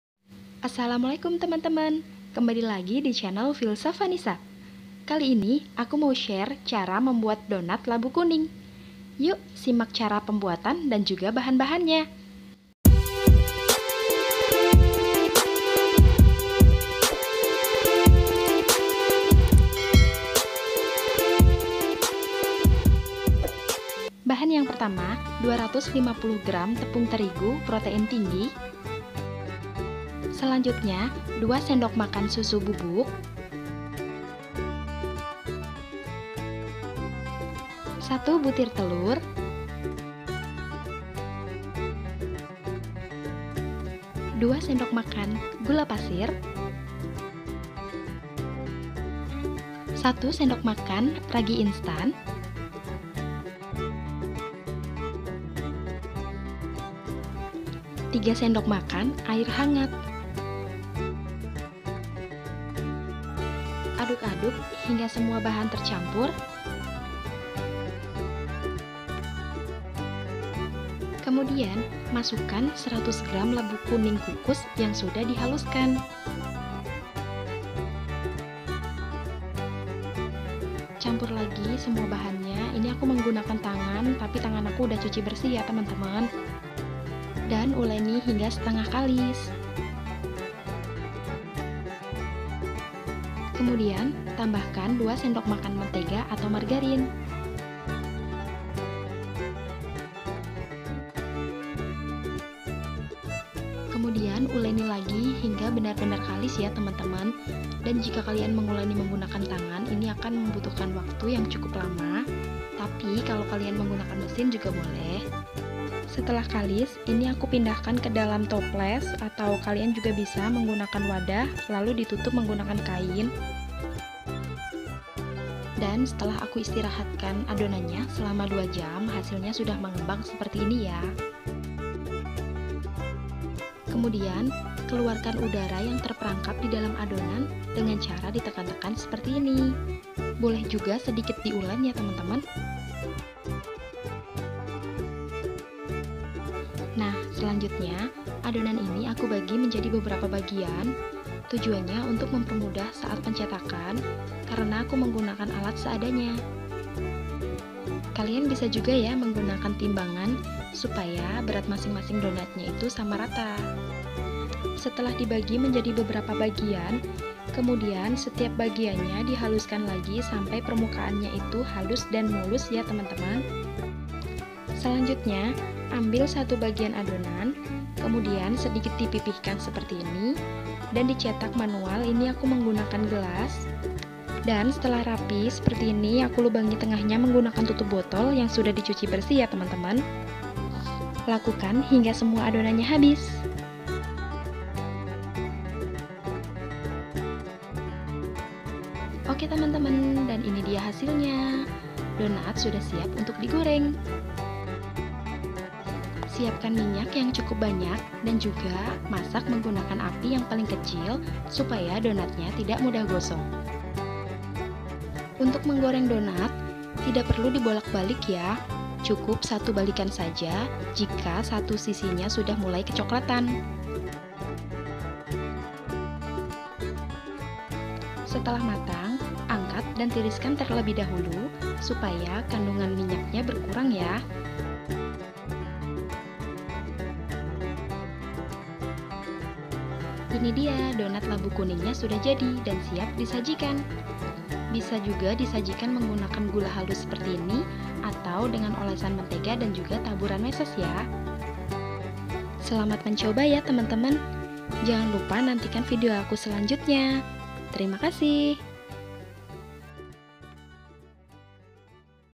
Resep Donat Labu Kuning Empuk Hanya 1 Telur
Assalamu'alaikum teman-teman, ketemu lagi dichannel Vilsafa nissa
Kali ini aku mau berbagi resep membuat donat labu kuning yang empuk hanya menggunakan 1 telur saja tetapi hasilnya mengembang sempurna
Bisa juga untuk ide jualan ya teman-teman
langsung saja ini bahan-bahannya

bahan donat:
- 250 gr tepung terigu pro tinggi
- 100 gr labu kukus dihaluskan
- 1 butir telur
- 2 sdm susu bubuk
- 2 sdm gula pasir
- 2 sdm mentega
- 3 sdm air hangat
- 1 sdm ragi instan

Resep ini menghasilkan 26 pcs donat
mohon maaf teman-teman bentuk donatnya masih kurang rapi hehe
cara membuatnya tonton video sampai habis ya teman-teman

Semoga bermanfaat dan terima kasih

Follow juga instagram @Vilsafa_nissa untuk update resep yang lainnya